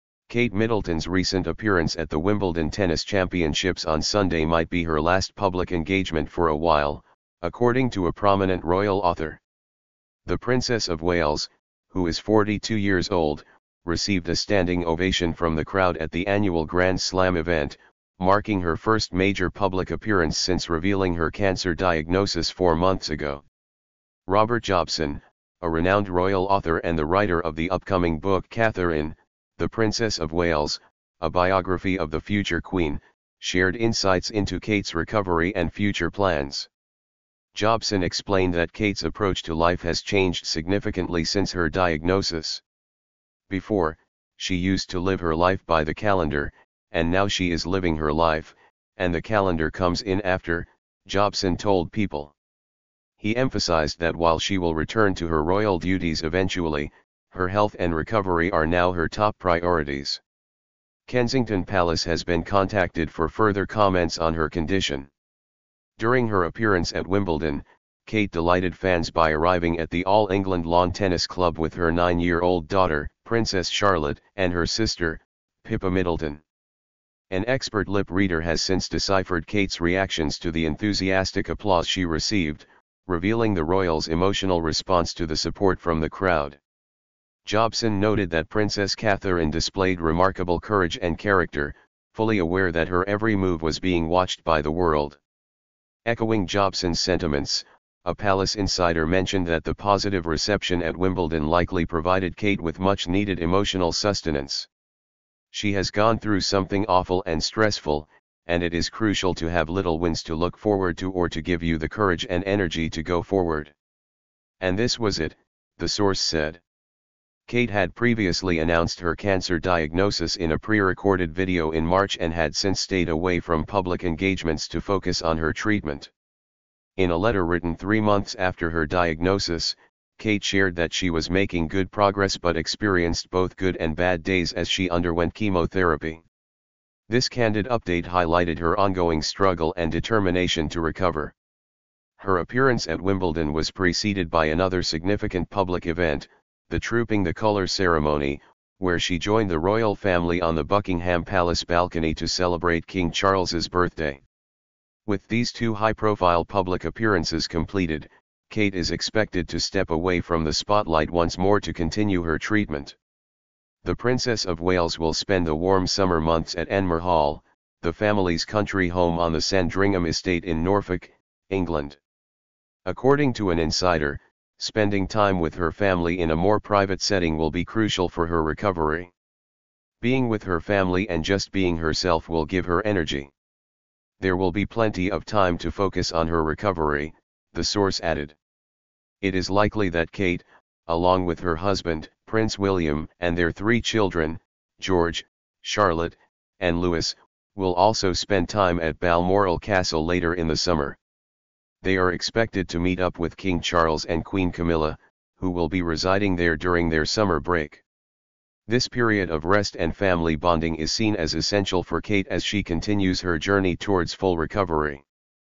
Kate Middleton’s Wimbledon appearance is likely last public outing for foreseeable future: author
Kate Middleton made a notable appearance at the Wimbledon Tennis Championships, but it might be her last public outing for a while. Renowned royal author Robert Jobson reveals that this appearance could be her final major public engagement in the near future due to ongoing health concerns. After her recent cancer diagnosis, Kate’s focus has shifted to her recovery, and this visit to Wimbledon was a significant moment for her and her fans. Join us as we discuss the implications of Jobson's insights and what this means for the Princess of Wales's future public appearances. Stay updated on Kate Middleton’s journey and royal family news right here!